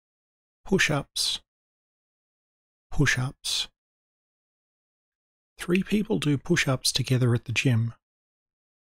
Push ups: English word pronunciation - Read Along (Australian Accent) with a picture
Three people do push ups together at the gym.

Please let me know if you have any requests for other words to make videos like this of.